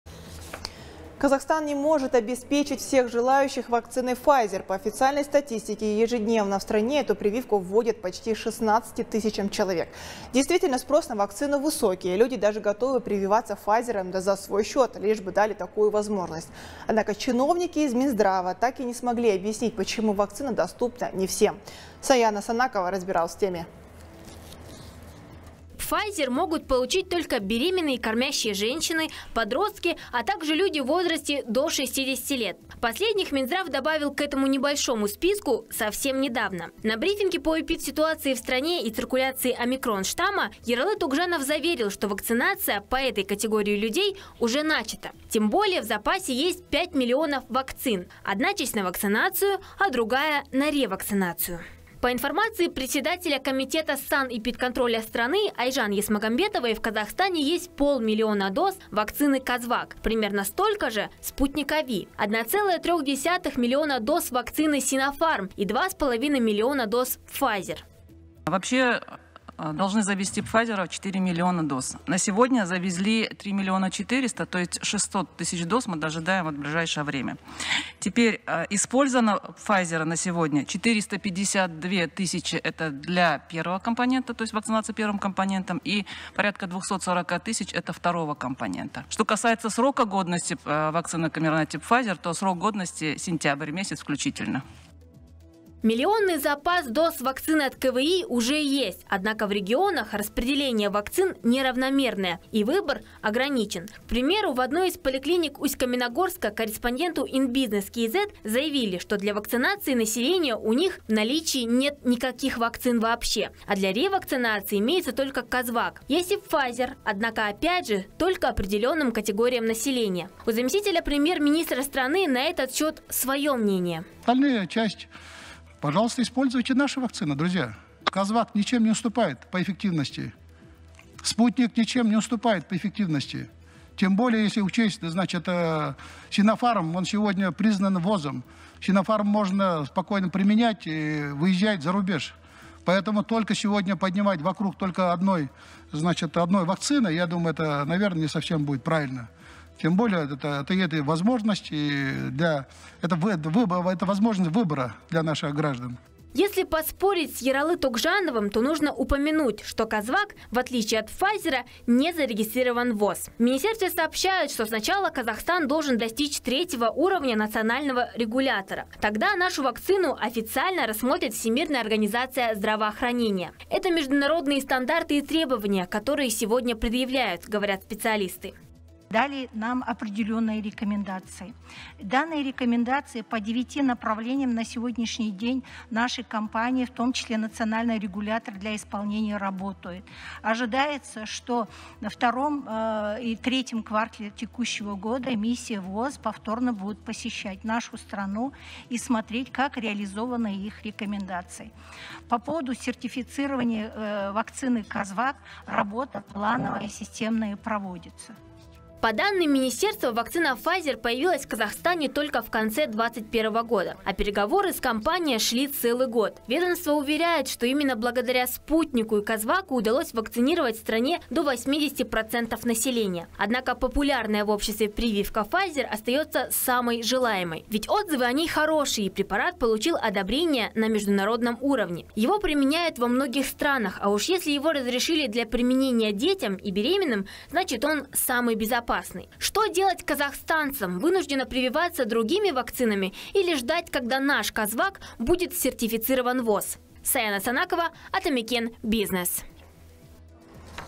ПОЧЕМУ PFIZER НЕ ДЛЯ ВСЕХ КАЗАХСТАНЦЕВ?
Казахстан не может обеспечить всех желающих вакциной Pfizer. По официальной статистике, ежедневно в стране эту прививку вводят почти 16 тысячам человек. Действительно, спрос на вакцину высокий, люди даже готовы прививаться Pfizer-ом за свой счет, лишь бы дали такую возможность. Однако чиновники из Минздрава, так и не смогли объяснить - почему вакцина доступна не всем. Саяна Санакова разбиралась в теме.